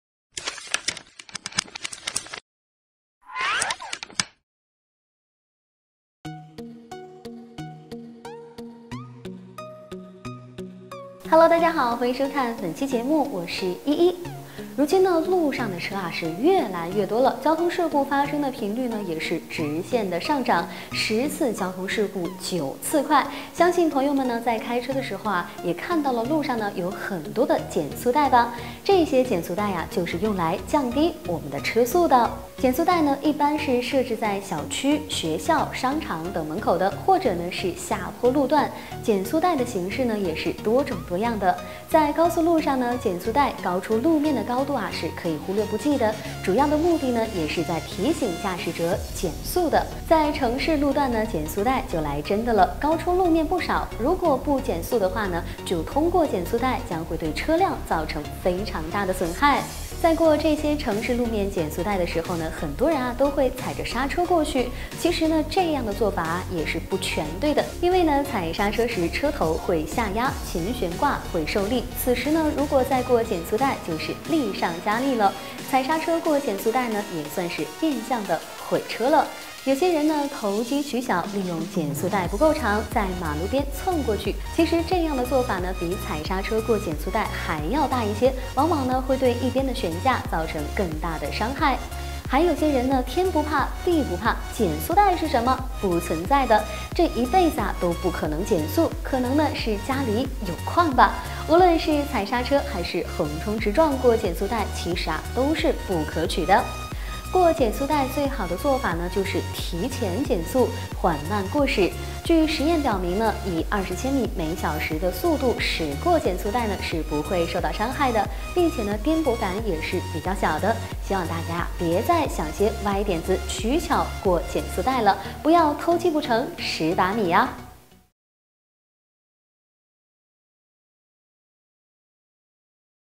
踩剎車過減速帶其實是變相毀車？用這一招讓你的愛車不怕“顛簸”
如今，路上的車越來越多，交通事故發生的頻率直線上漲。十次交通事故九次快，相信朋友們在開車的時候也看到了路上有很多減速帶吧，這些減速帶就是用來降低我們的車速。